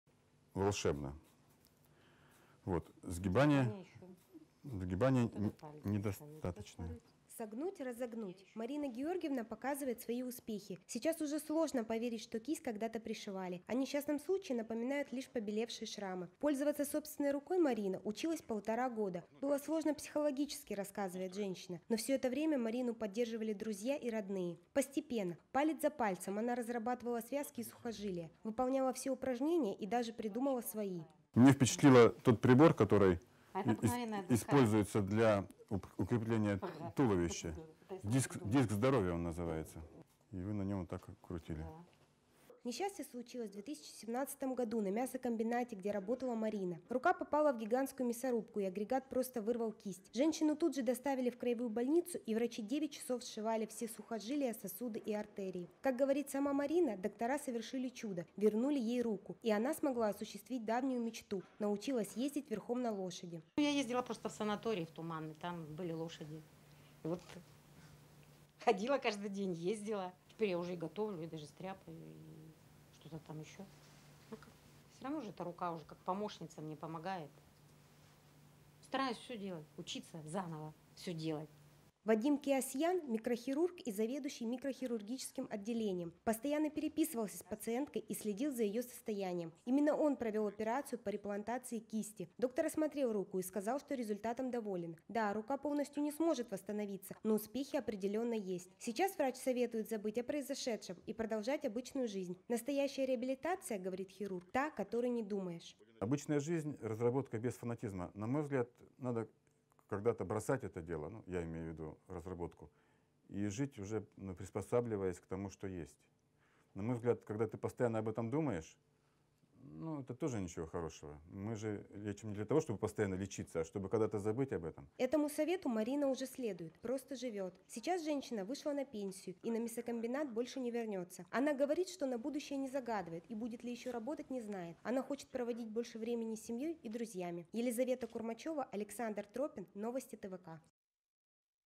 Женщина с пришитой рукой научилась писать (Красноярск)
Красноярские хирурги рассказали о сложном случае: женщина полностью лишилась кисти в результате несчастного случая на производстве. Во время ЧП коллеги сориентировались − спасли оторванную часть руки и отправили пострадавшую в Краевую больницу.

Прошло меньше года со сложной 9-часовой операции и женщина сегодня почти полностью реабилитировалась.